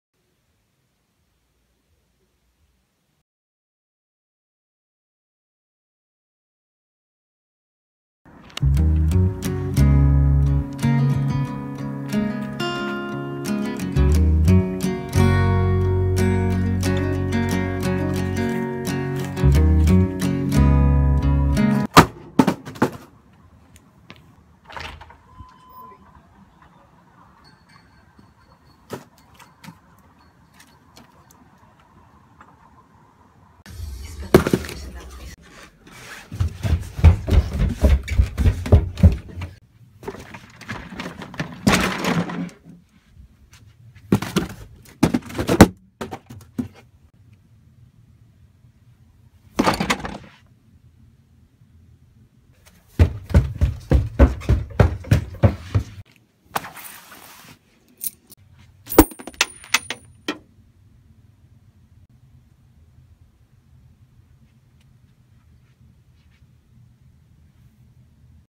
A New Chair!?
Yesterday I received a package with my new chair!! and I thought I would make this funny little skit about it. :)